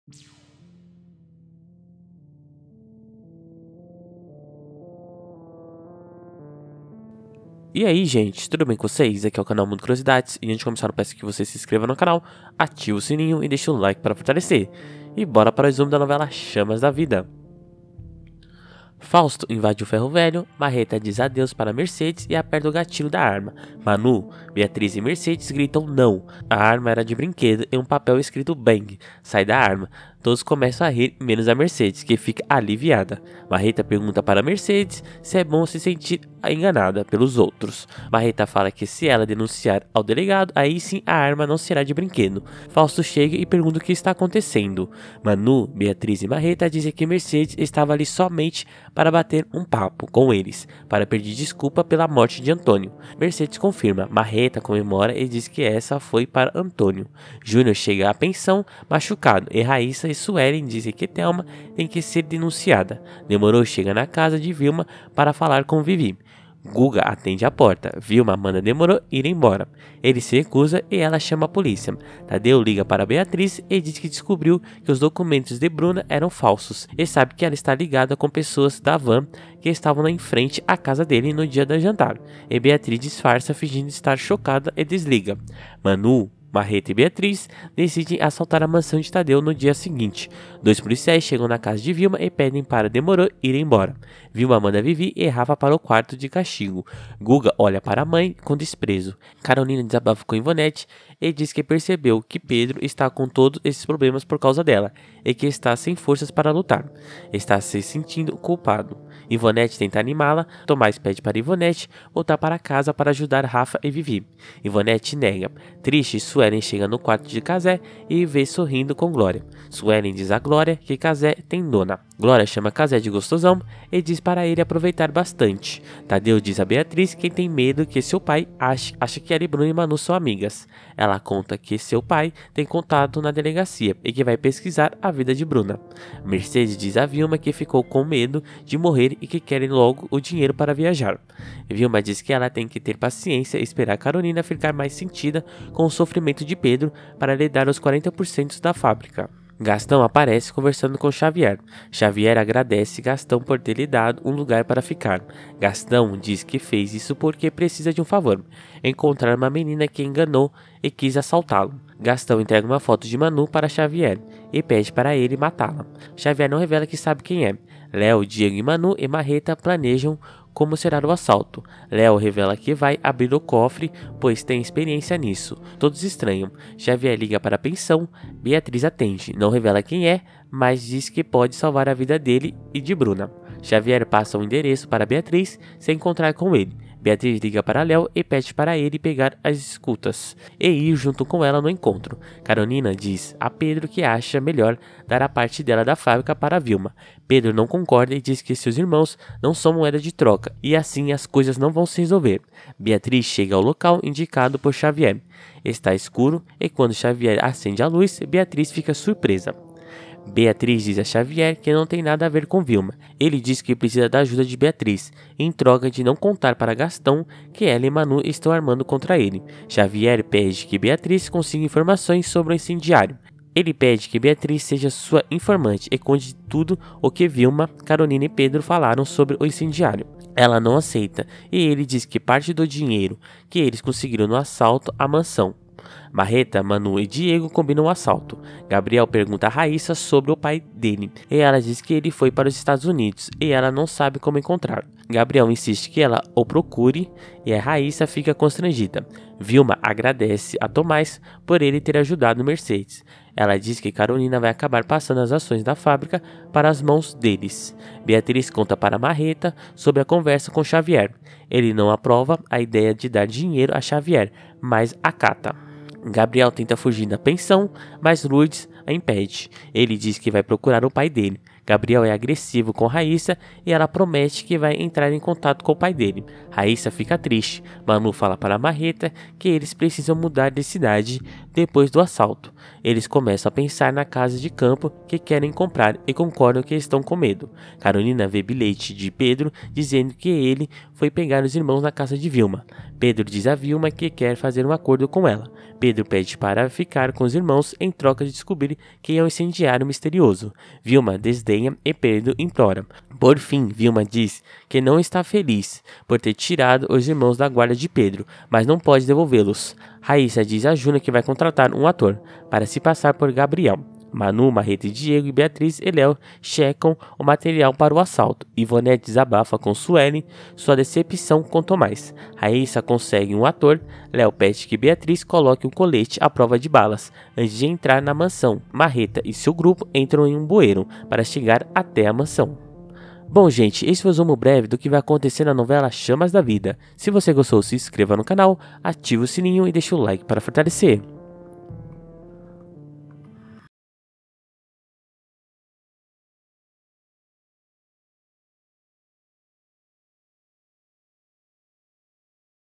CHAMAS DA VIDA-CAPITULO DE QUINTA-FEIRA 29.09.22 - RESUMO DE HOJE DA NOVELA CHAMAS DA VIDA!
CHAMAS DA VIDA-CAPITULO DE QUARTA-FEIRA 29.09.22 - RESUMO DE HOJE DA NOVELA CHAMAS DA VIDA!

Capítulo 154, quinta, 29 de setembro

Fausto invade o ferro velho. Marreta diz adeus para Mercedes e aperta o gatilho da arma. Manu, Beatriz e Mercedes gritam “não!”. A arma era de brinquedo e um papel escrito “bang” sai da arma. Todos começam a rir, menos Mercedes, que fica aliviada. Marreta pergunta para Mercedes se é bom se sentir enganada pelos outros. Marreta fala que se ela o denunciar ao delegado, aí sim, a arma não será de brinquedo. Fausto chega e pergunta o que aconteceu. Manu, Beatriz e Marreta dizem que Mercedes estava ali somente para bater um papo com eles para pedir desculpa pela morte do Antonio. Mercedes confirma. Marreta comemora e diz que essa foi para Antonio. Junior chega à pensão machucado e Raíssa e Suelen dizem que Telma tem que ser denunciada. Demoro chega na casa de Vilma para falar com Vivi. Guga atende a porta. Vilma manda Demoro ir embora, ele se recusa e ela chama a polícia. Tadeu liga para Beatriz e diz que descobriu que os documentos de Bruna eram falsos e que sabe que ela está ligada com o pessoal da van que estava em frente a casa dele no dia do jantar. Beatriz disfarça, finge estar chocada e desliga.

CANAL DE NOTICIAS,